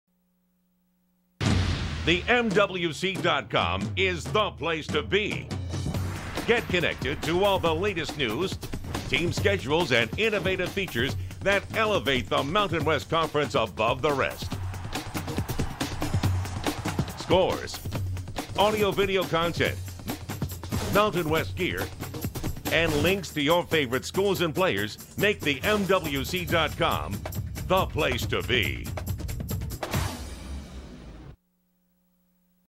MWC Reach Your Peak – TheMWC.com
For this project, I was the Creative Director and Copywriter for a branding campaign and a series of TV Ads for the Mountain West Conference. We created the tagline "ABOVE THE REST" to position the MWC as one of the top athletic conferences in the country. The tagline had to be relevant to the Mountain West region and its member schools, and it had to be applicable to both academics and athletics while conveying a positive image for the conference, its student athletes and its fans.

The campaign theme "REACH YOUR PEAK" was chosen because of its aspirational nature that connects student athletes and fans with the unique geographic region that is home to all of the conference's member universities.